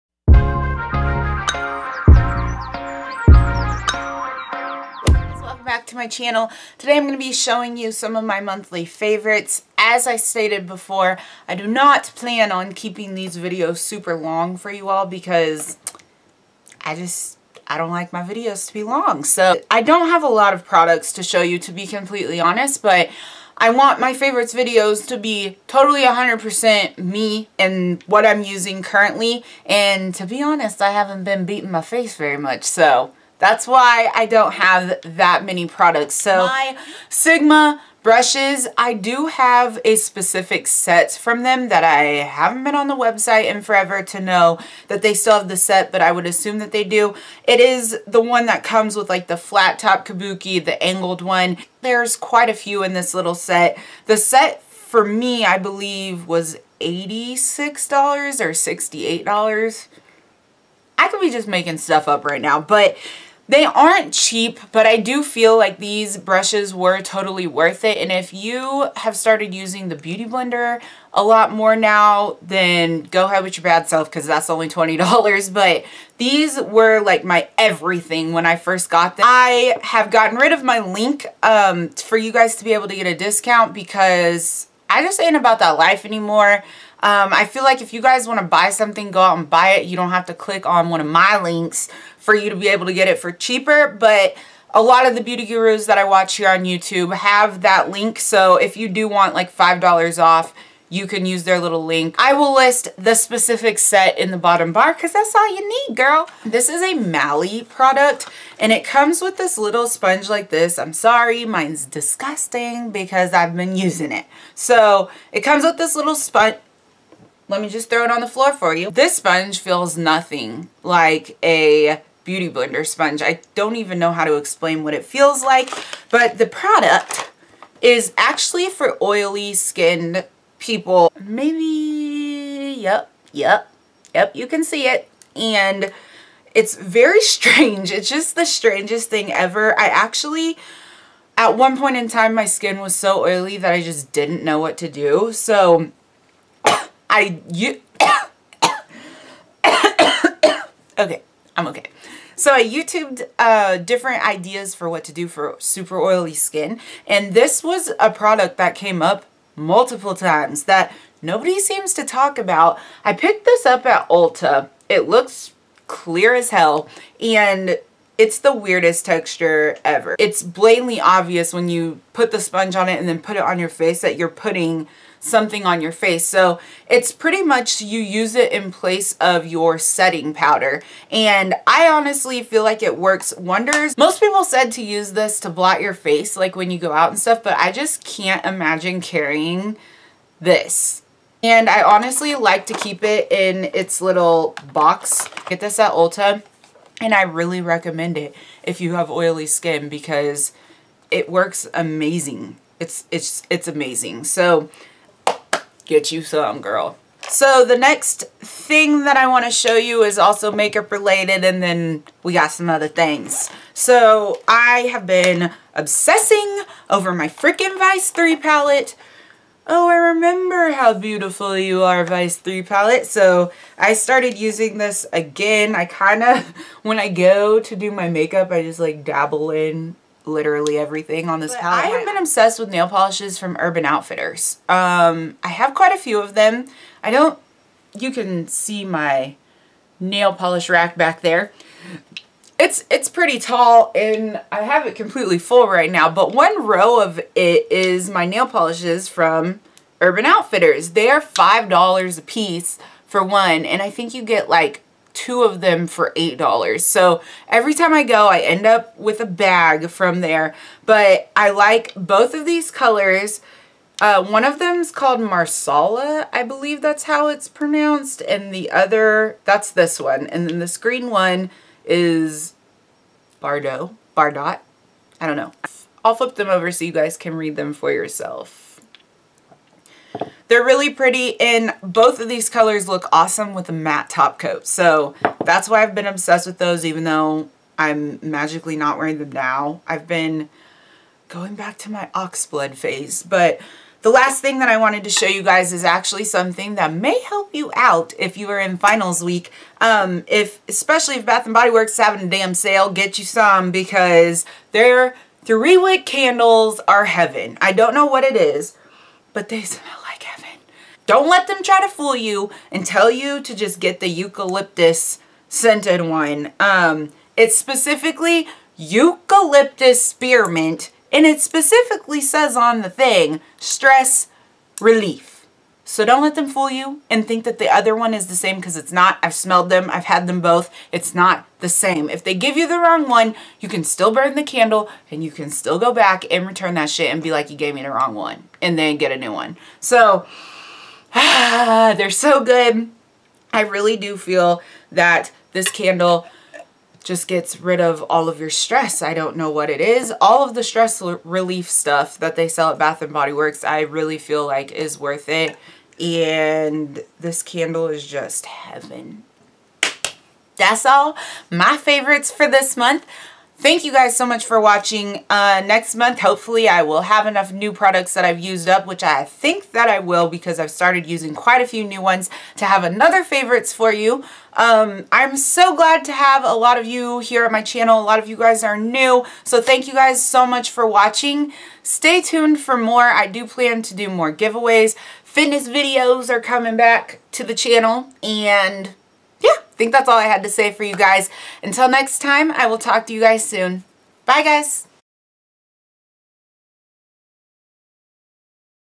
Monthly Favorites | November 2015
VIDEO FAQ BELOW.  UPGRADE TO HD FOR BETTER QUALITY.

1. Sigma Makeup Brushes
    - Flat Kabuki F80
    - Round Kabuki F82
    - Angled Kabuki F84
    - Angled Kabuki F88
    - Tapered Kabuki F86

2. Mally Beauty Evercolor Powerless Face Defender
3. Urban Outfitters Nail Polish
4. Bath and Body Works Aromatherapy Stress Relief Candle
    - Eucalyptus Spearmint Three Wick Candle

~Filmed with: Canon HR 400
~Edited with: iMovie '11